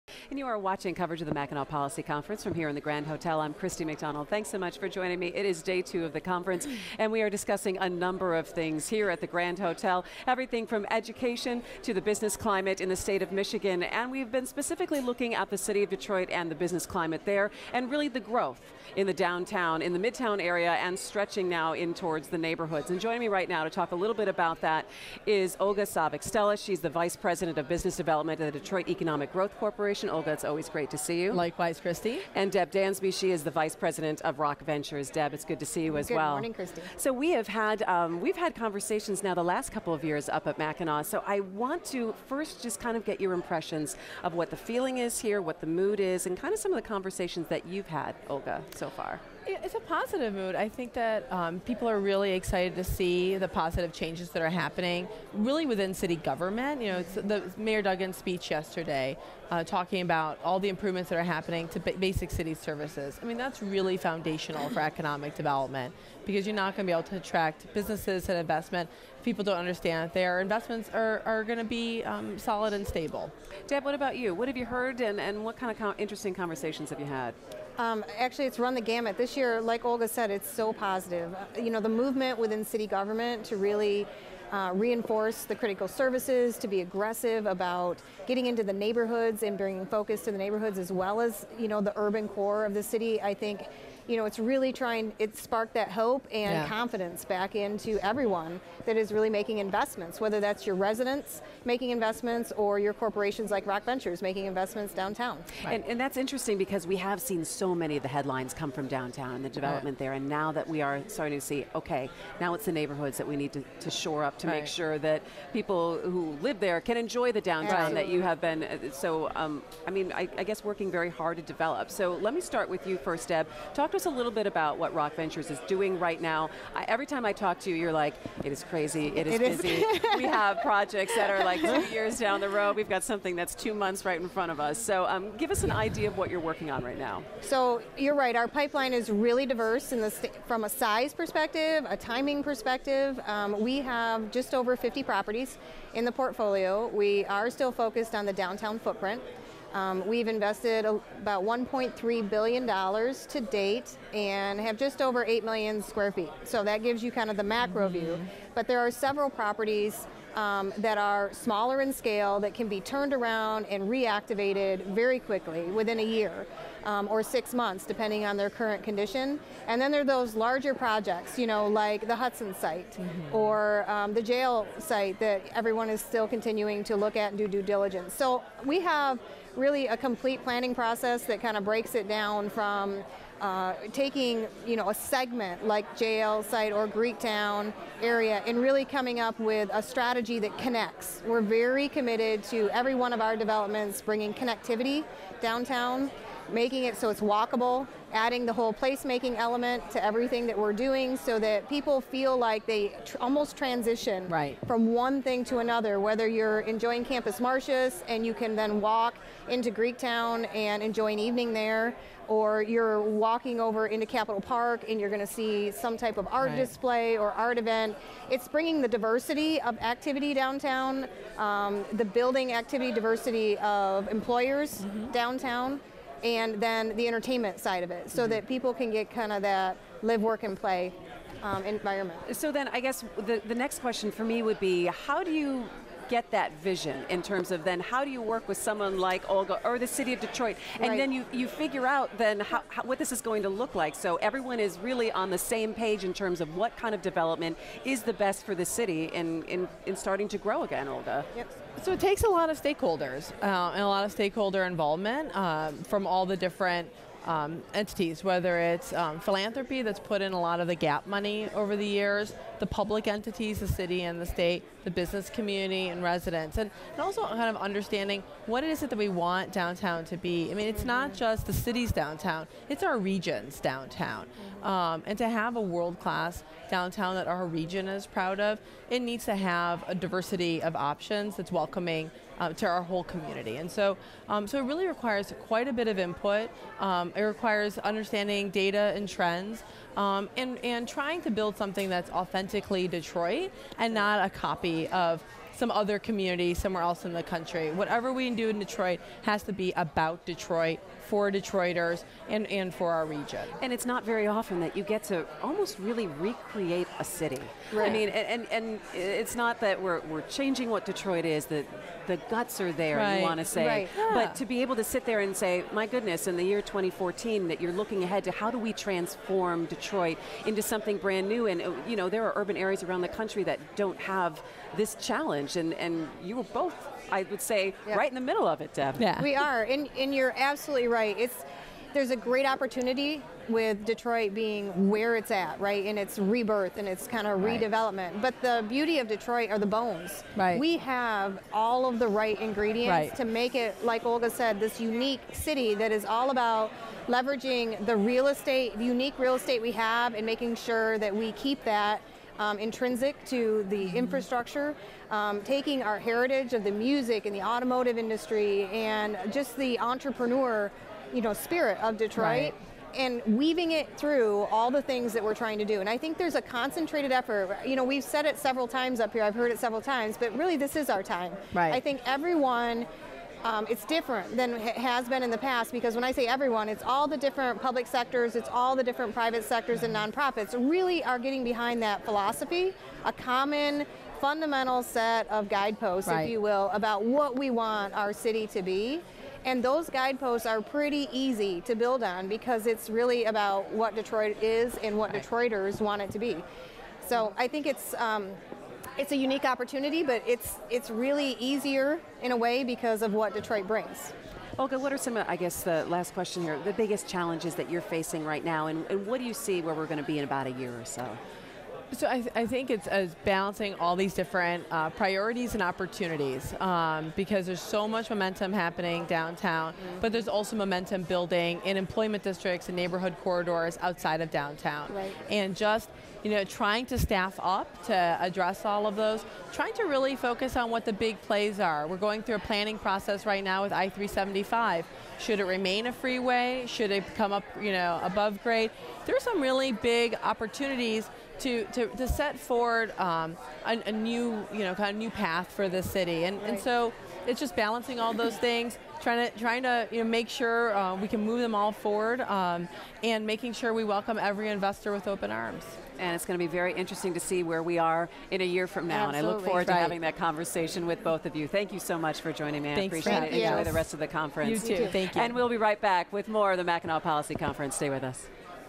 Olga Stella and Deb Dansby Interview - MPC 14
Christy talks with Olga Stella and Deb Dansby about growth and corporate investment in downtown Detroit.
Olga Stella is VP of Business Development at Detroit Economic Growth Corporation, and Deb Dansby is Vice President of Rock Ventures, LLC.